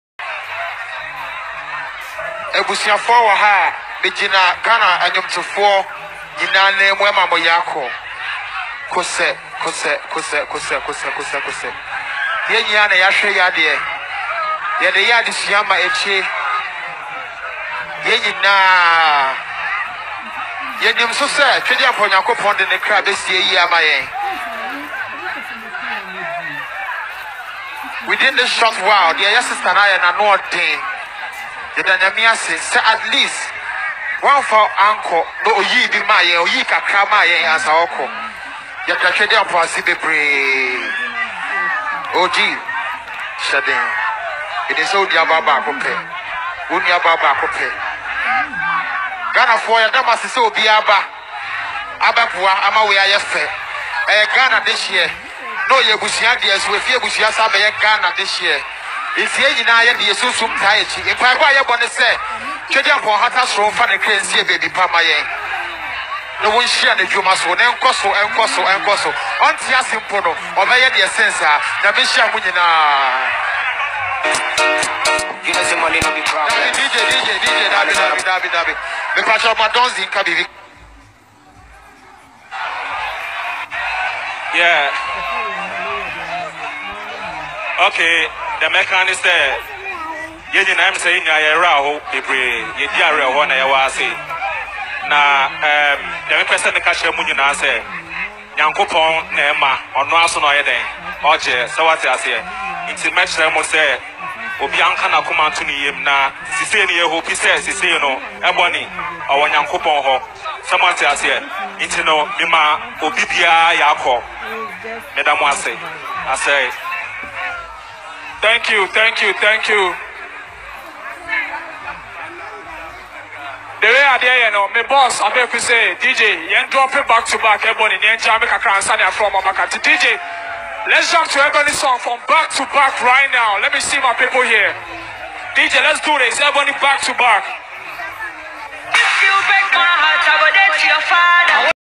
Sarkodie’s emotional speech at Ebony’s one week celebration
MentiasieTV